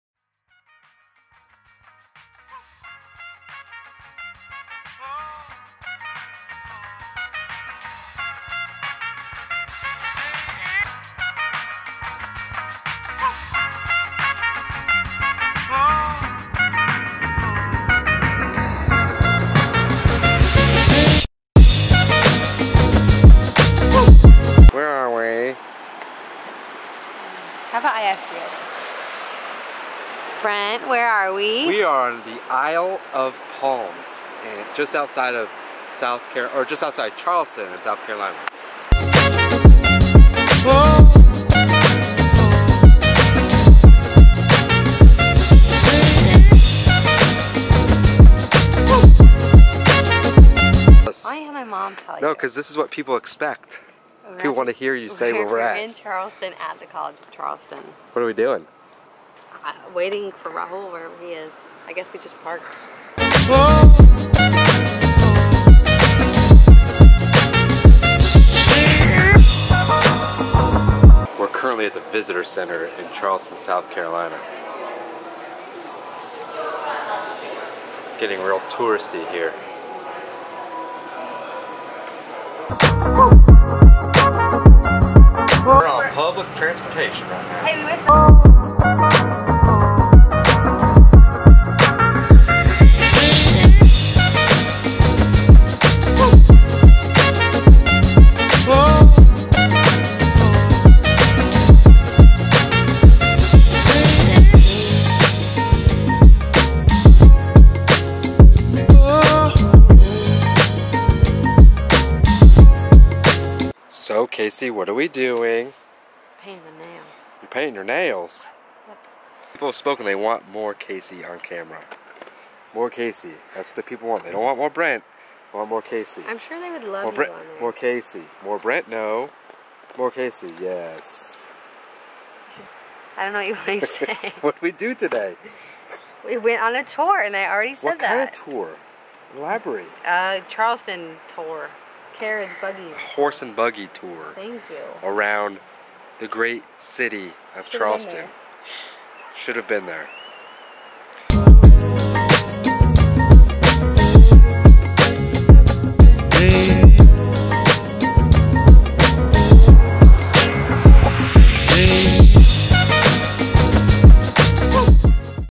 A Quick Weekend Trip to Charleston, SC
We took a quick weekend trip to Charleston, South Carolina.